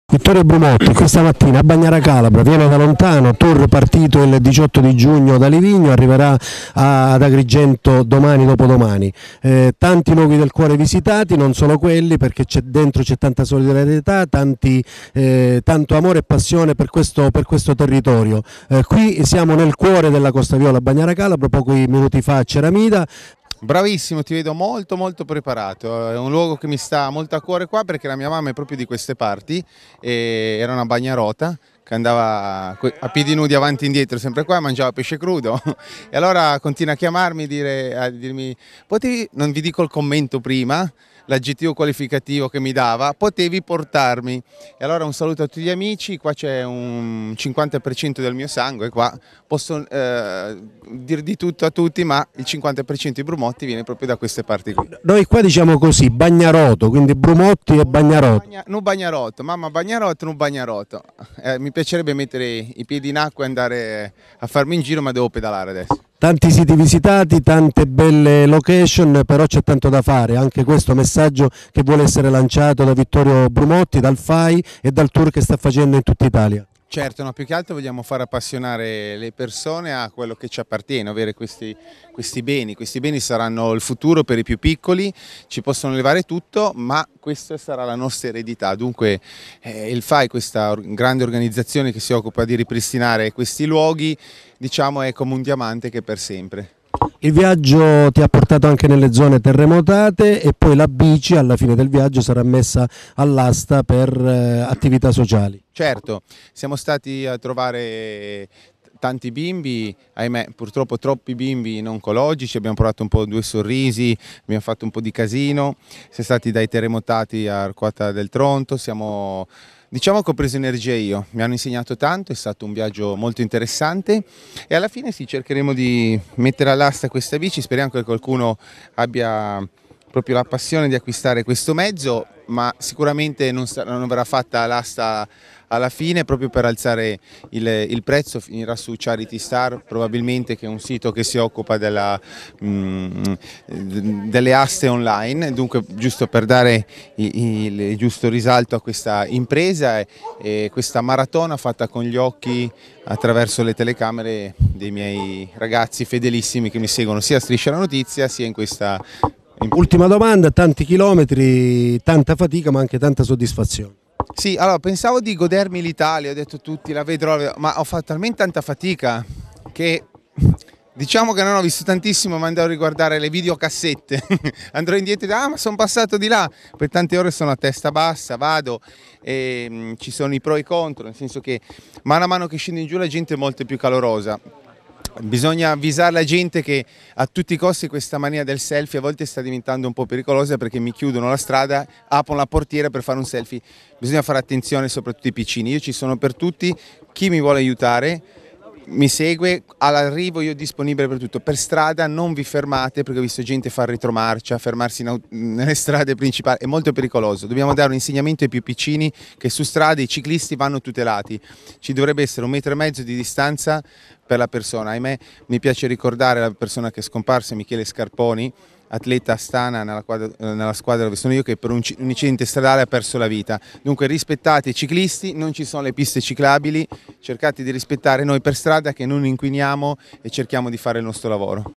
TAPPA A BAGNARA CALABRA PER IL TOUR DI VITTORIO BRUMOTTI
Altra tappa calabrese per Vittorio Brumotti. Il suo tour in giro per l'Italia è patrocinato dal FAI - Fondo Ambiente Italiano e attraversa diversi Comuni e borghi di tutta l’Italia.